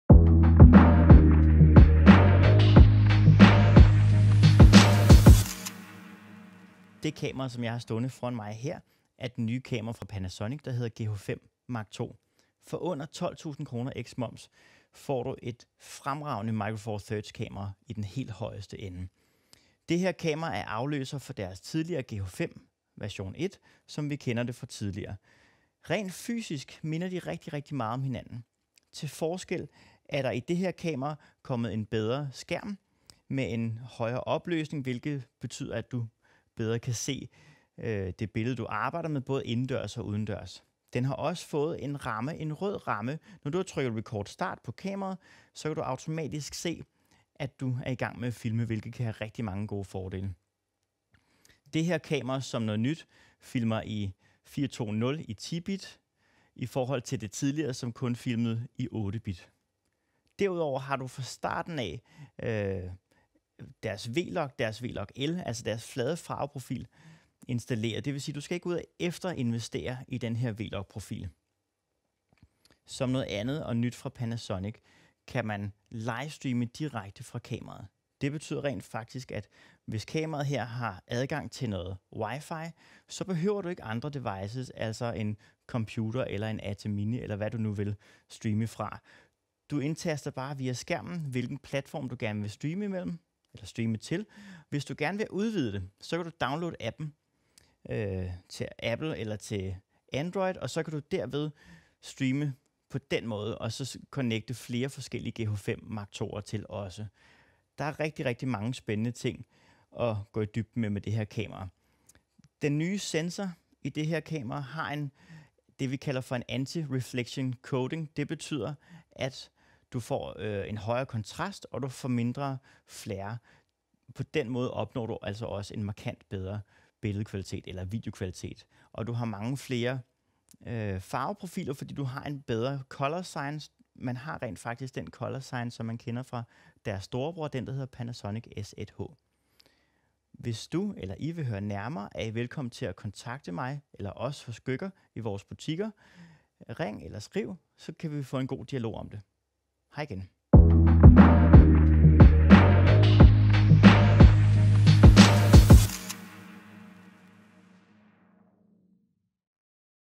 Præsentation af Panasonic GH5 II
Her får du en kort dansk præsentation af det nye Panasonic GH5 II hybridkamera.